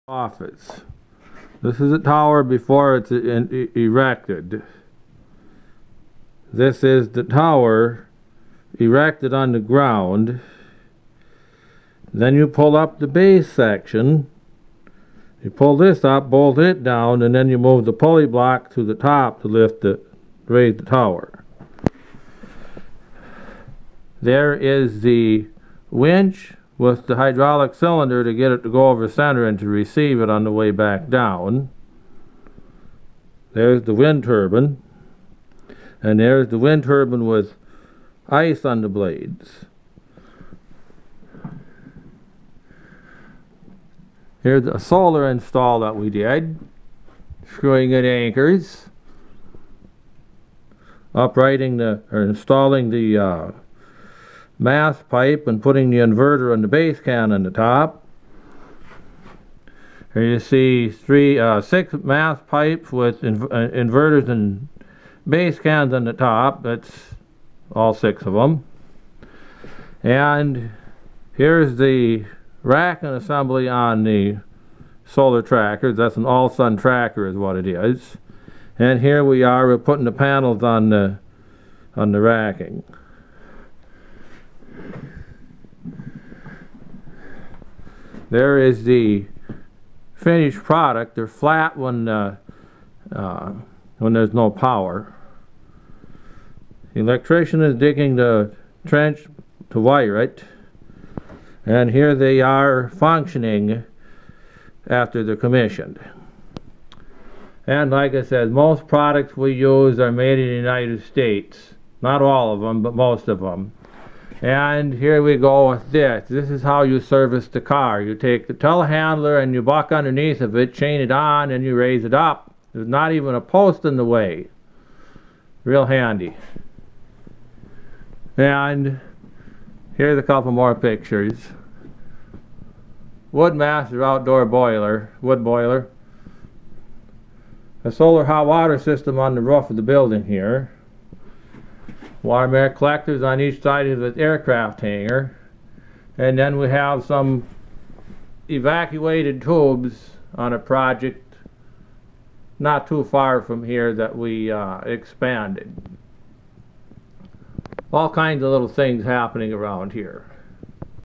Pics in office of some solar and wind turbine projects
Wind turbine folding tower from start to finish, all sun trackers install start to finish, Woodmaster out door wood boiler, solar skies solar Collectors, evacuated tube hit water collectors.